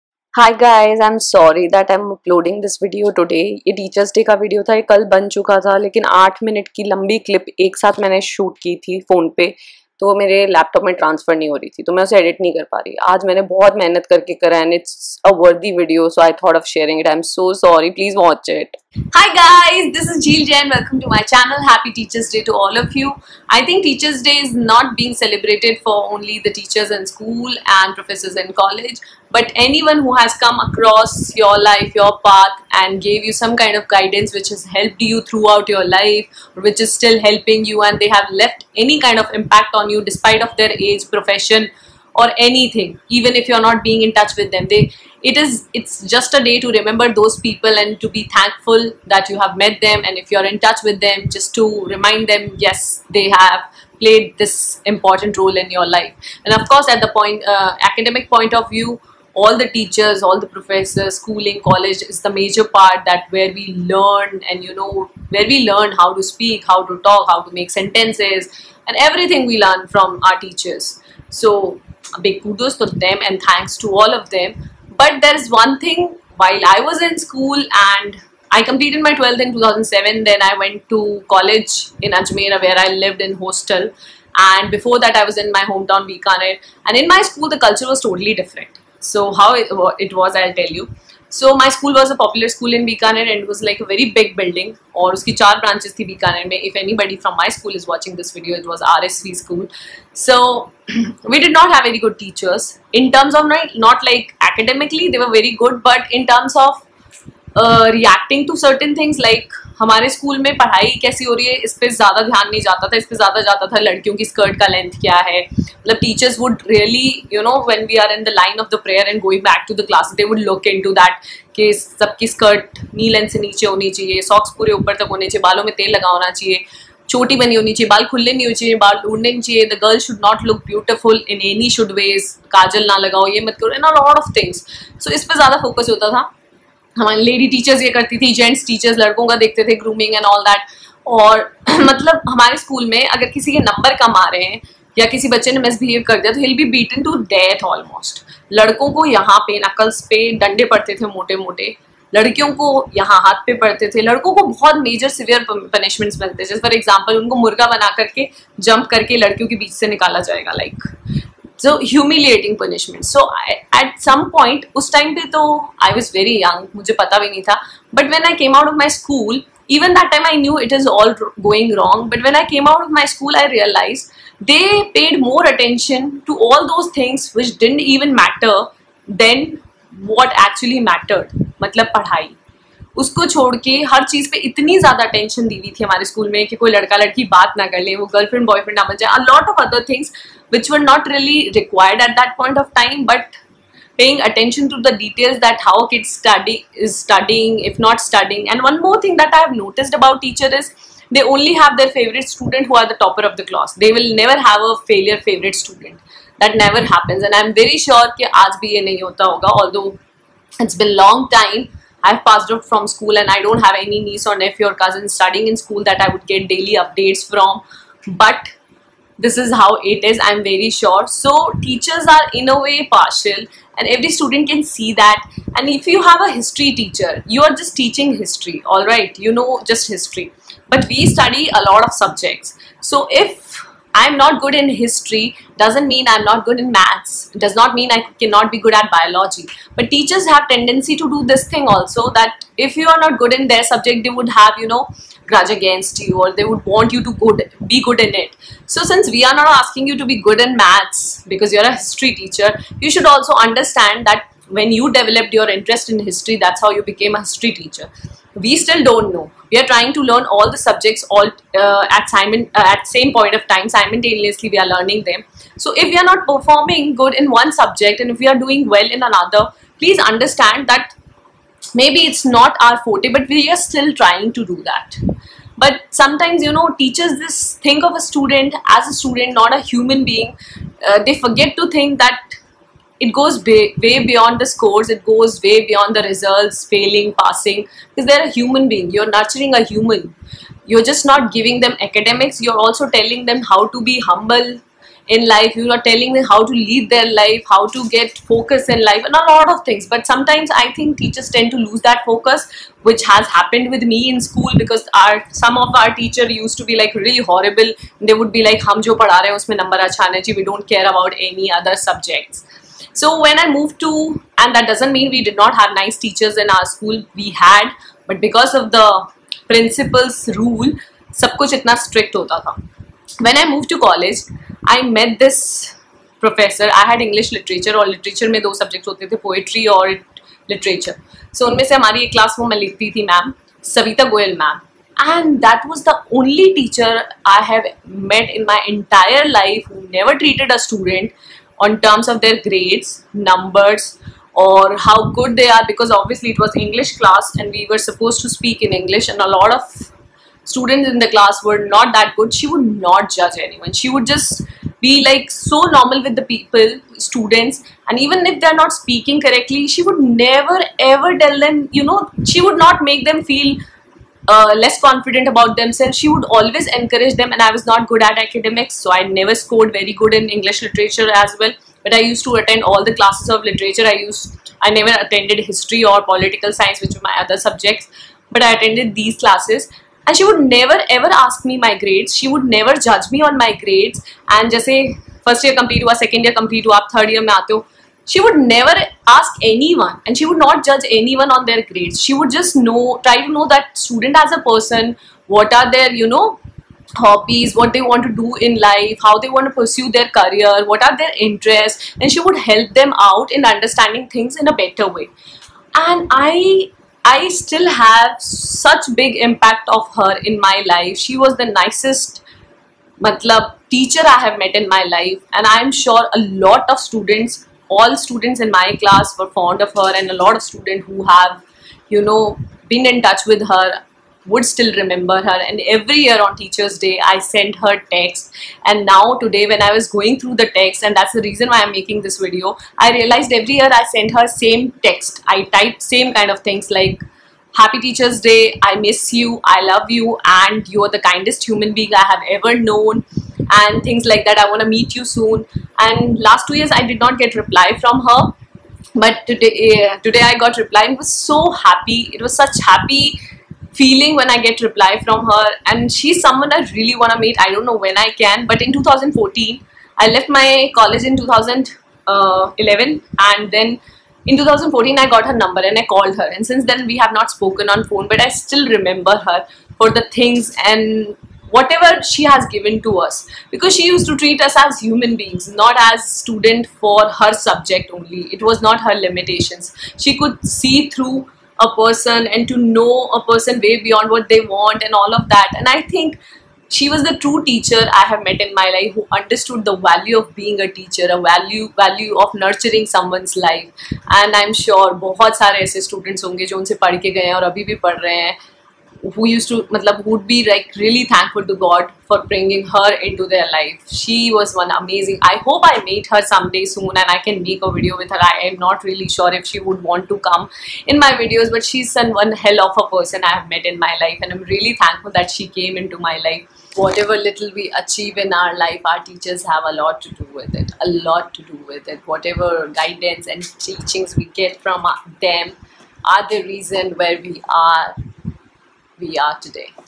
My Experience With Teachers In School & College | Teacher's Day Special
Little about my school and college